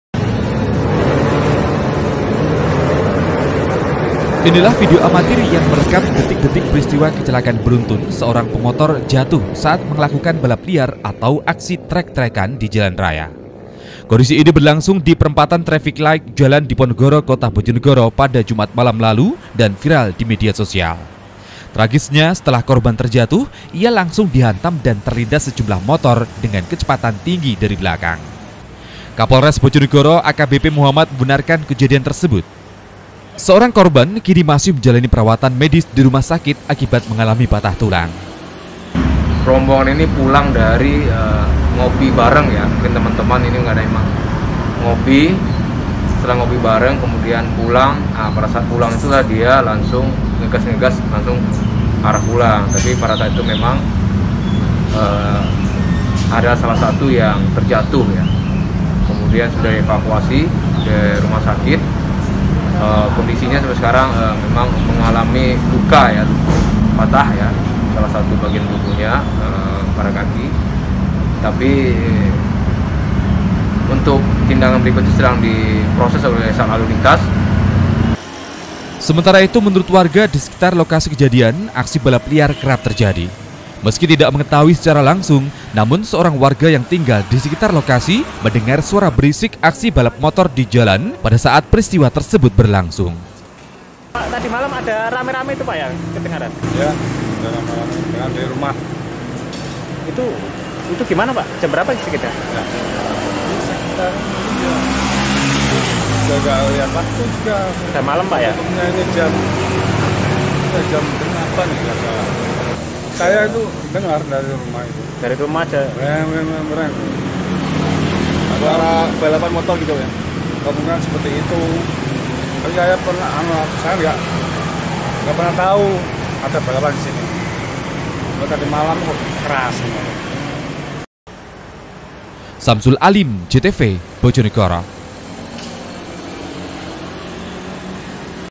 BOJONEGORO - Viral, Seorang Pemuda Jatuh Dan T3rl1nd4s Saat Balapan Liar
Diduga sedang melakukan balap liar atau aksi trek-trekan seorang pemuda jatuh di jalan protokol kota bojonegoro. Tragisnya korban yang jatuh sempat terlindas sejumlah motor dengan kecepatan tinggi dari belakang
Office : jl. Sarimulya no 47 Rejomulyo, Kartoharjo, Kota Madiun